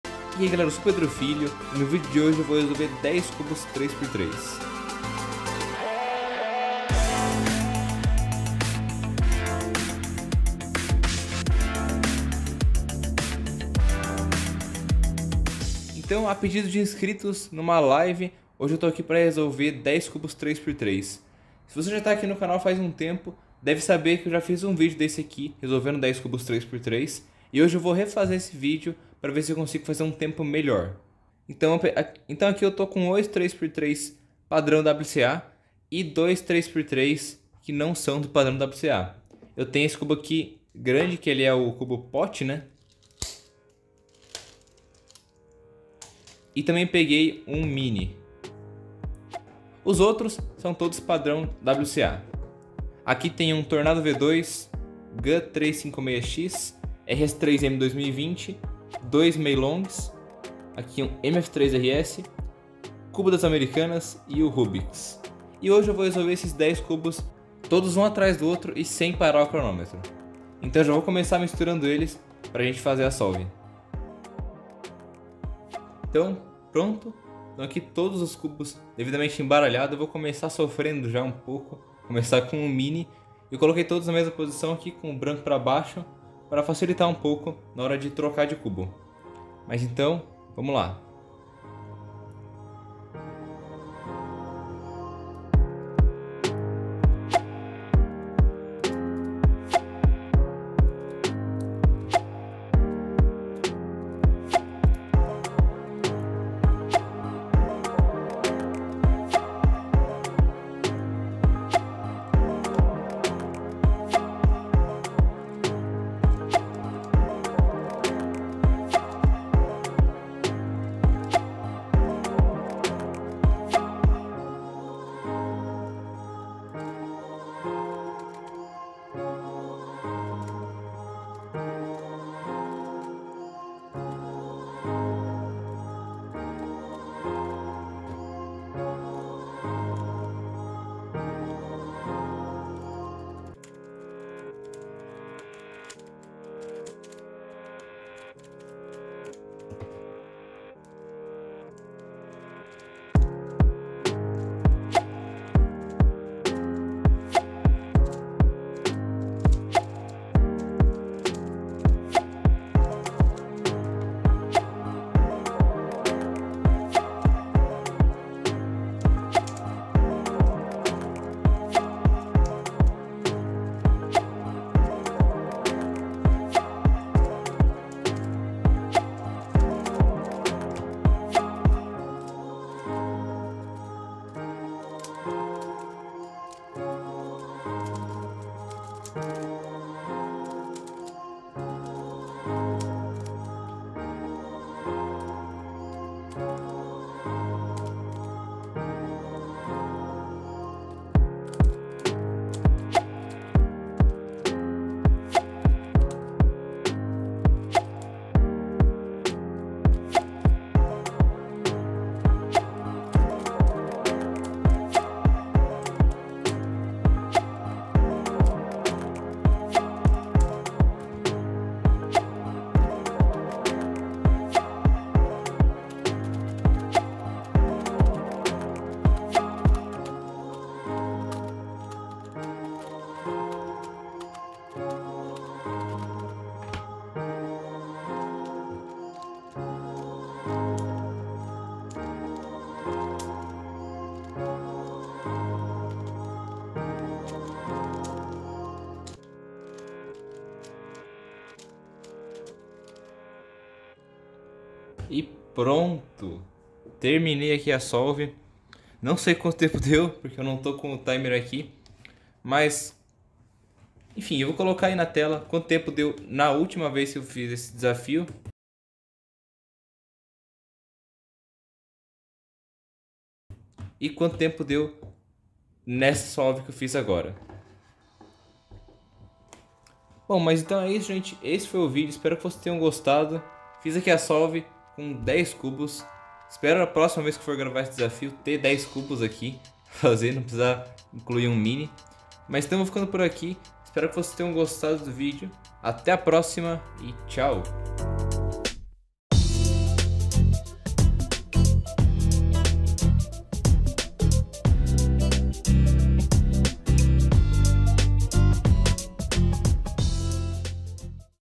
Resolvendo 10 Cubos 3x3 SEM PARAR
Hoje vou resolver 10 cubos 3x3 sem parar o cronômetro.

CUPOM: PEDROFILHO